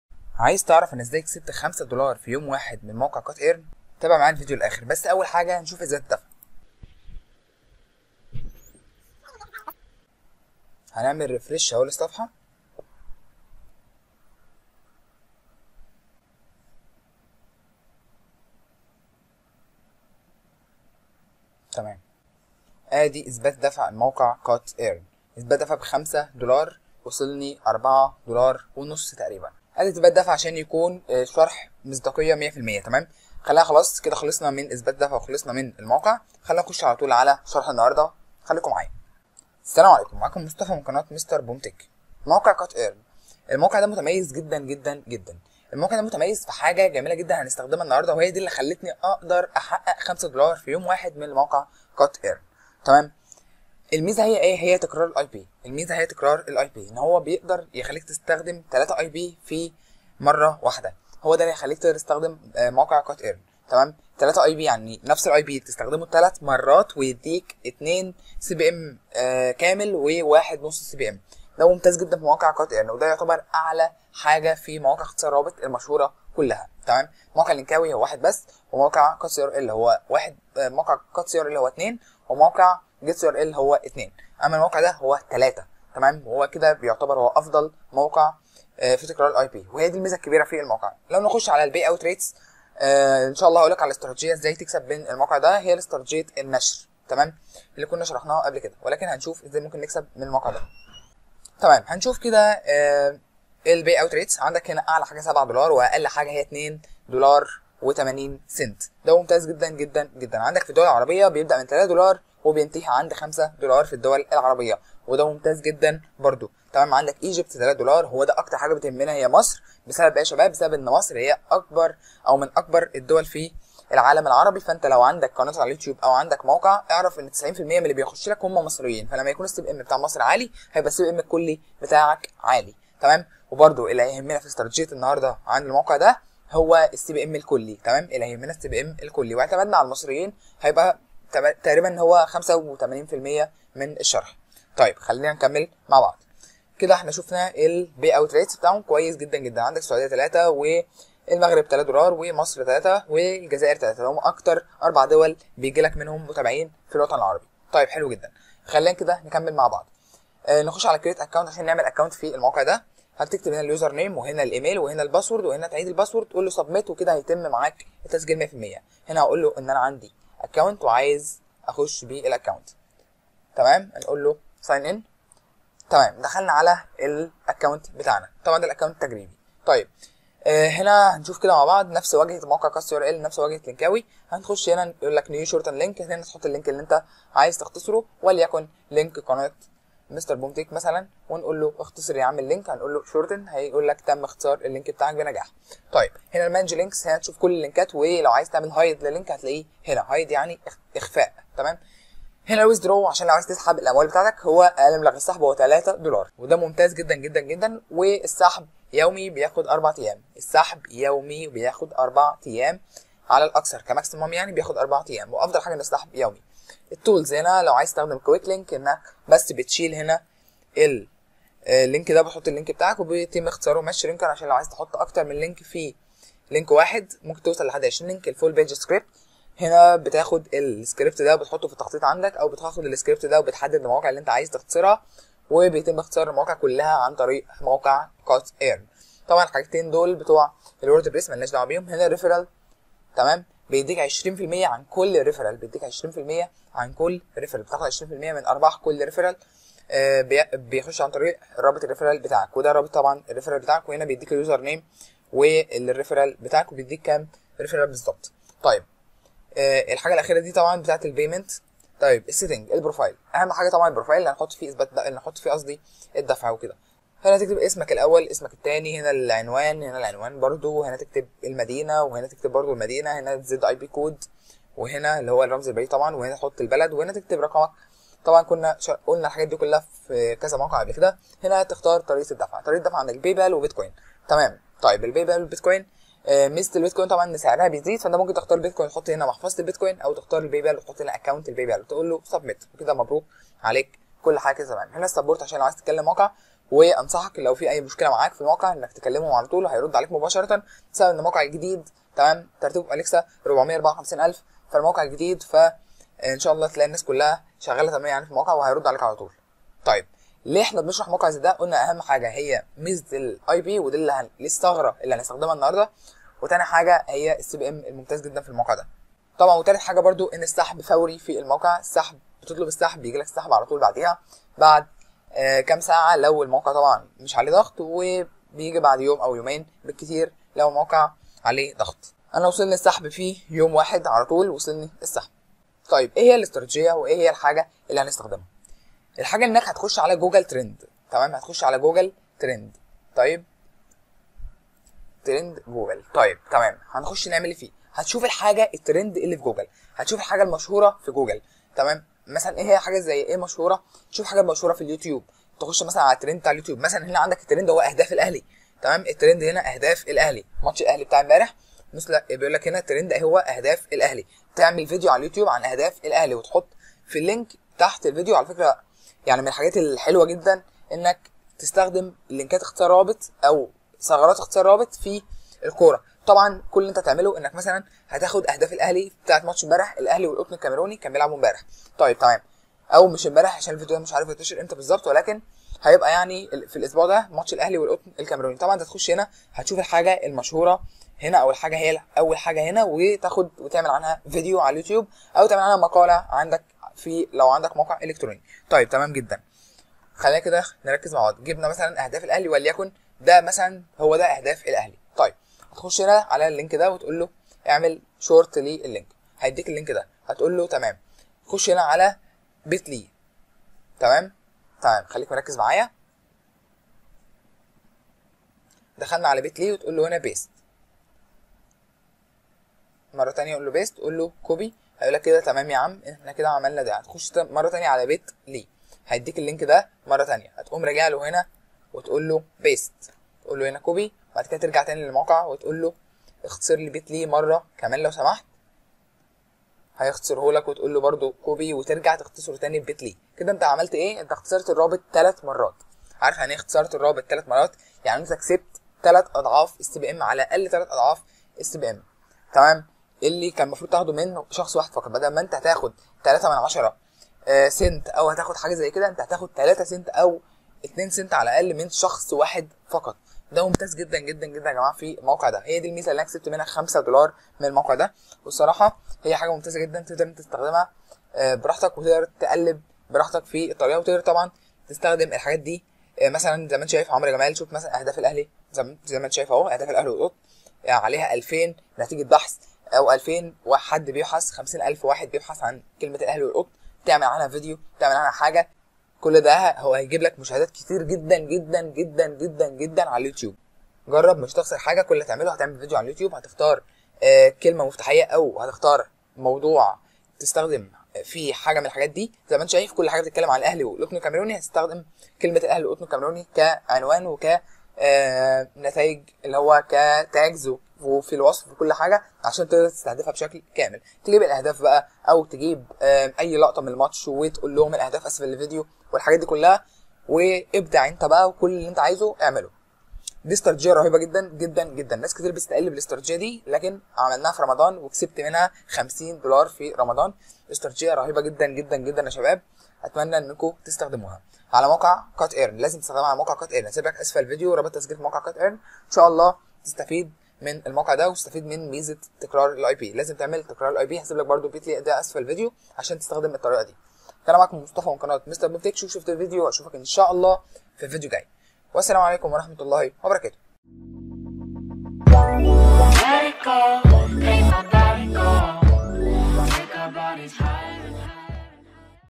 استراتيجية ربح 5 دولار اقل شئ يوميا من Cut Earn + اثبات الدفع !
سلام عليكم , في حلقة انهارده شرحت موقع Cut Earn وهو موقع جديد للربح من اختصار الروابط الموقع فيه بعض الميزات الجديده التي تم ذكرها في الفيديو و الحمدلله الموقع صادق و تم الدفع لي * و ارجو من الجميع عدم الاستهانة بالاستراتيجية التي تم شرحها في الفيديو متنساش تعمل فولو